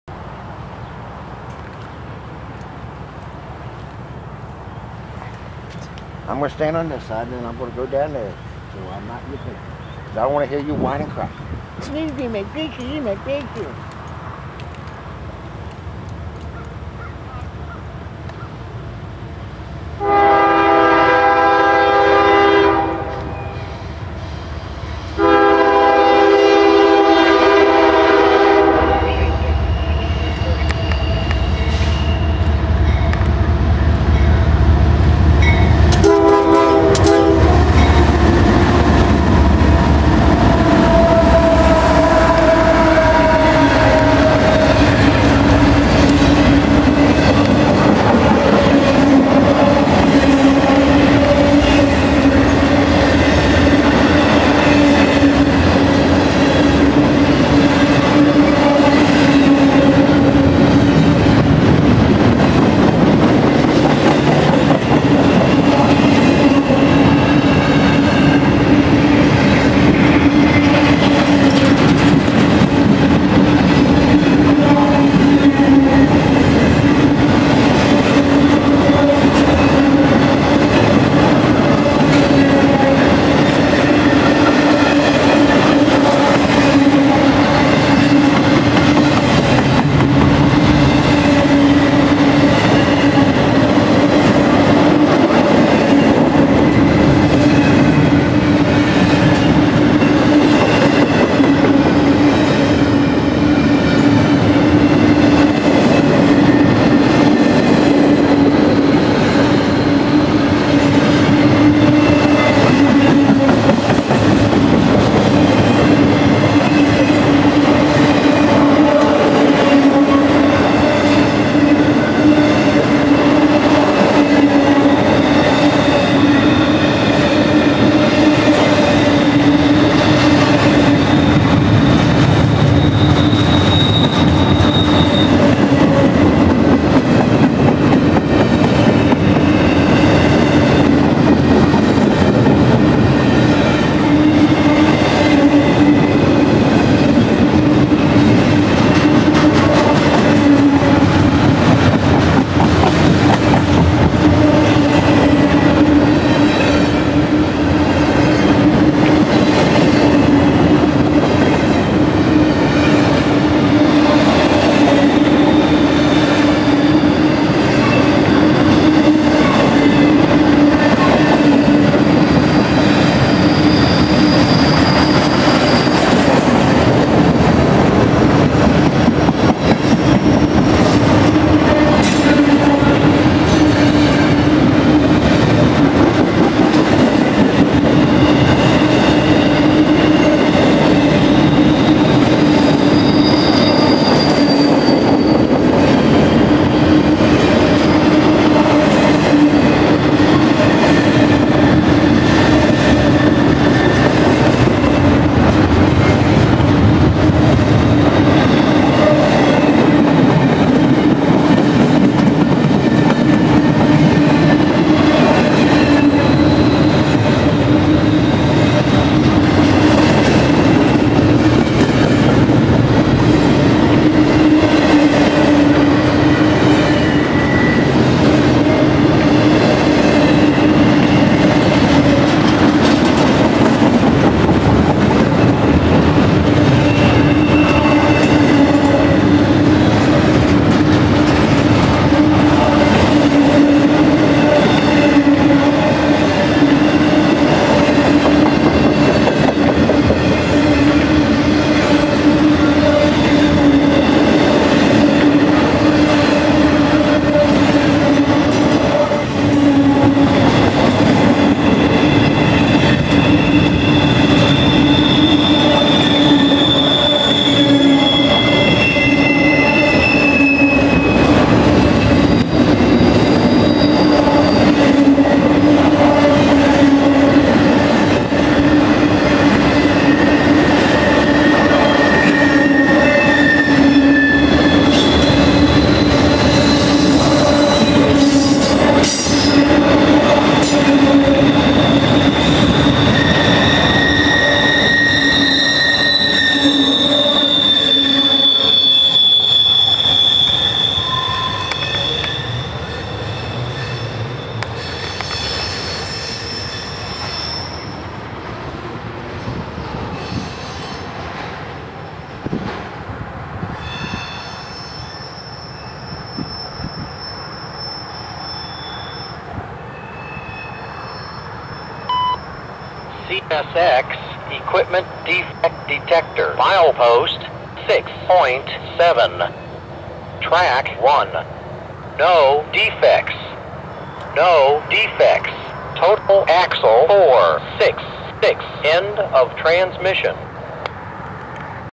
A 3 unit Q032 through st.denis
12,400 hp
7/30/17
12:18 pm
East on 1